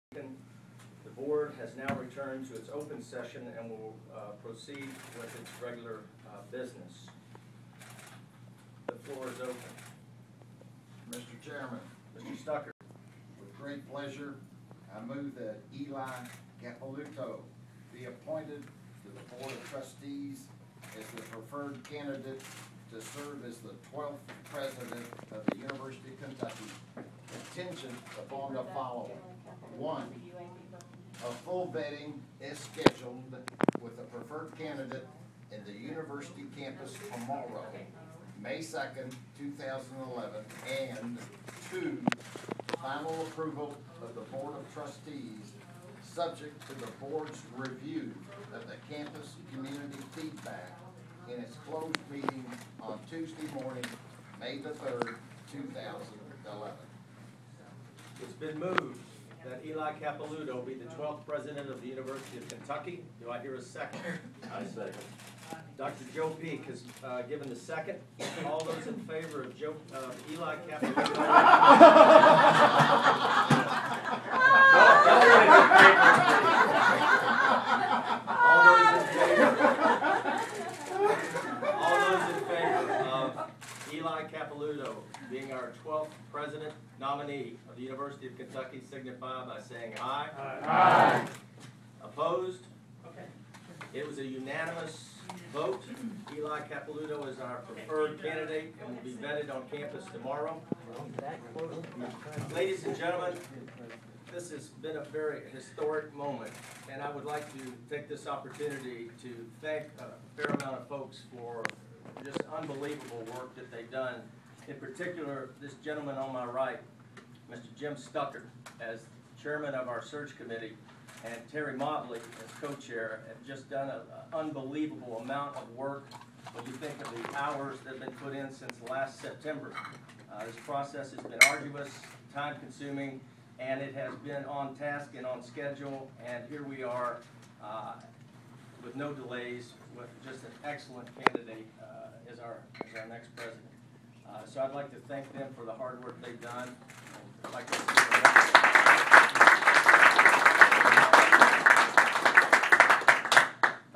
UK announces presidential pick
The presidential finalist for University of Kentucky has been named May 1, 2011. Eli Capilouto, the provost for The University of Alabama at Birmingham, was chosen as the Board of Trustees finalist for the 12th president of the University of Kentucky.